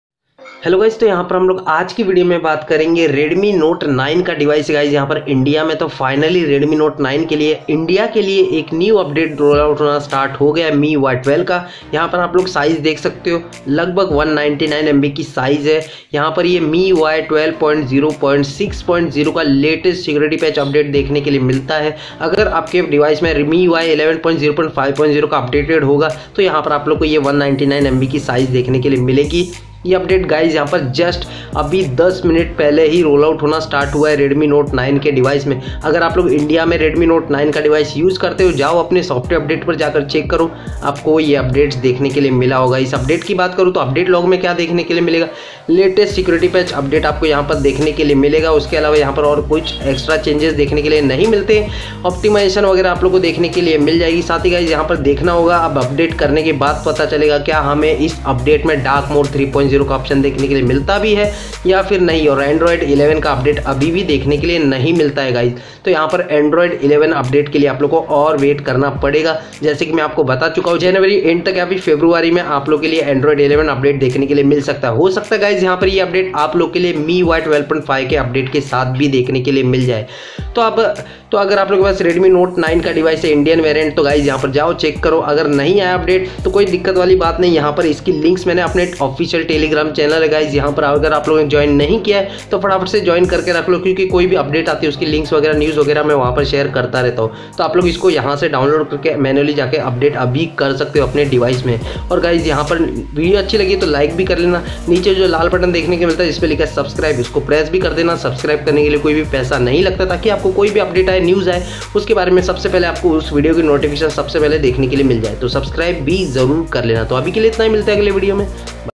Miui 12 New Update - Redmi Note 9 Android 11 | Redmi Note 9 Miui 12.0.6.0 Update
Aaj ki video me hum baat karenge android 11 update released ho gaya hai aur Redmi note 8 android 11 update, Redmi note 9 pro android 11 update, Redmi note 9 android 11 update, Redmi note 9 pro max android 11 update kab tak rollout hoga isliye video ko end tak dekhte rahna.

bhi sawal hai to comment karke batana

Please.

Redmi note 9 android 11 update